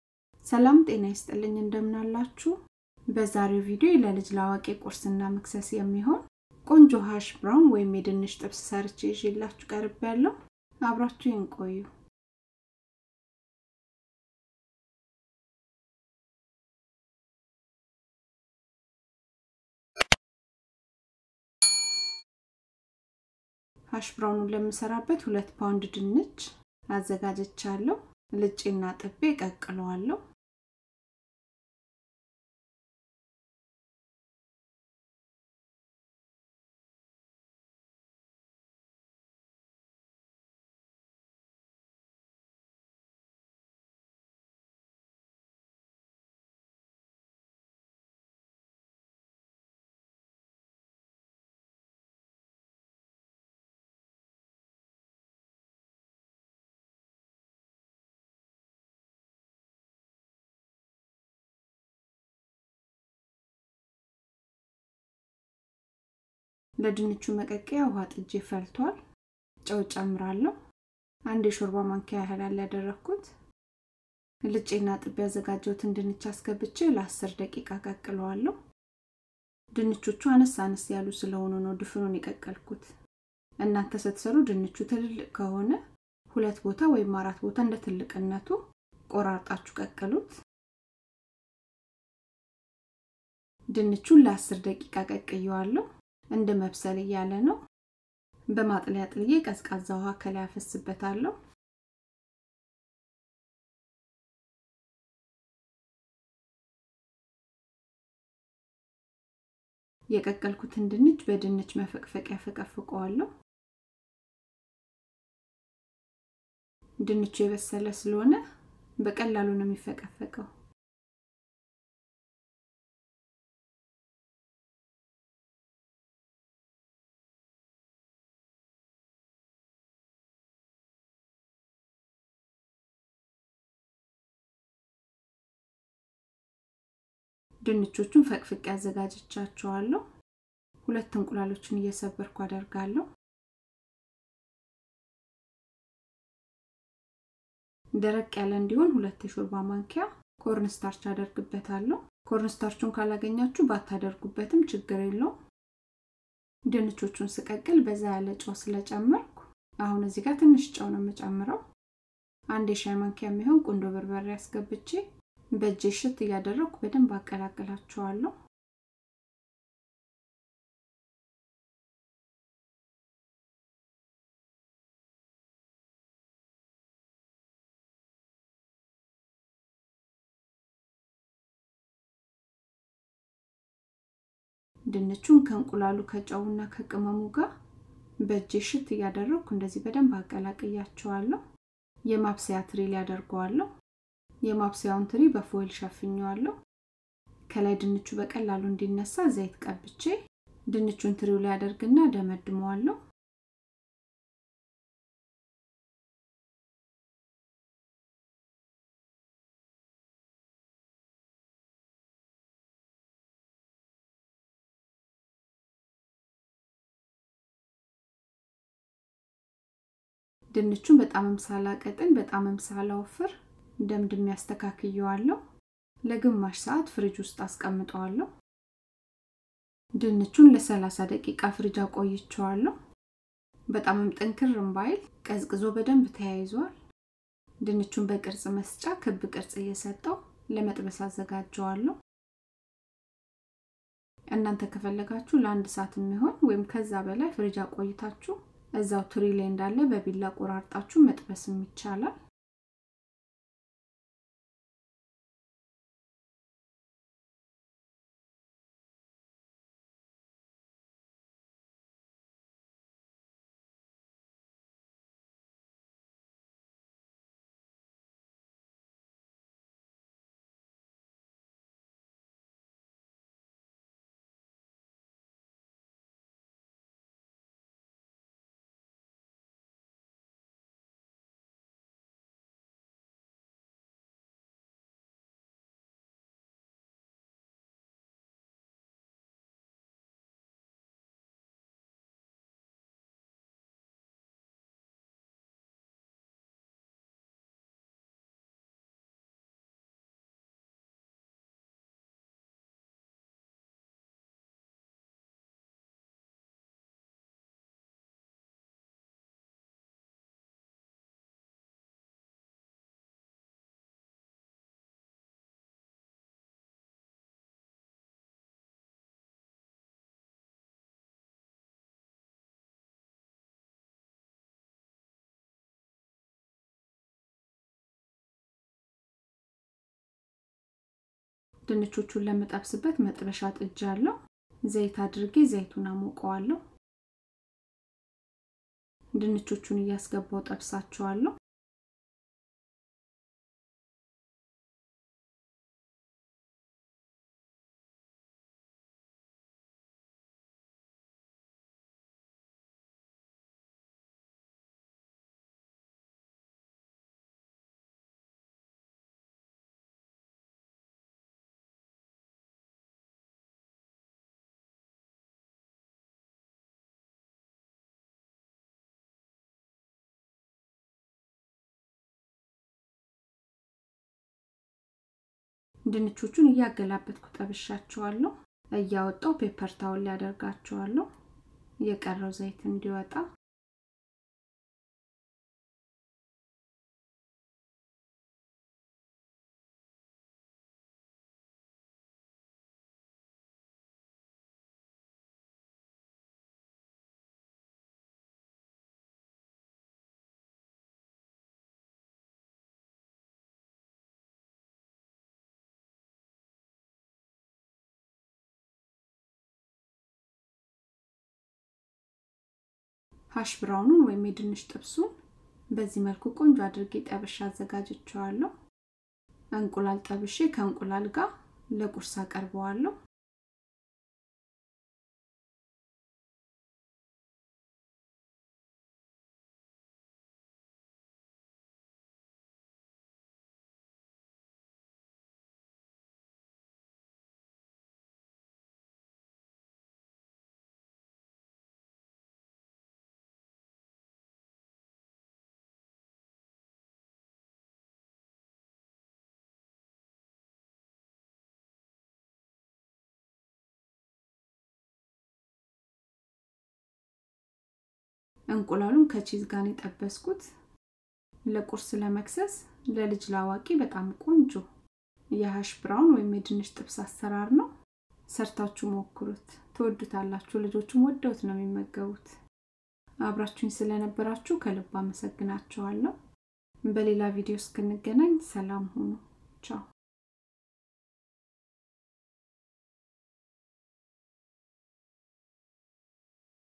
ለቁርስ የድንች ጥብስ (የሃሽ ብራውን አሰራር || Hash Browns
የድንች ጥብሱን (የሃሽ ብራውኑን)ለመስራት የተጠቀምኳቸው
* ድንች
* እንቁላል
* ኮርነስታርች
* ጨው
* ቁንዶበርበሬ
* ለመጥበሻ ዘይት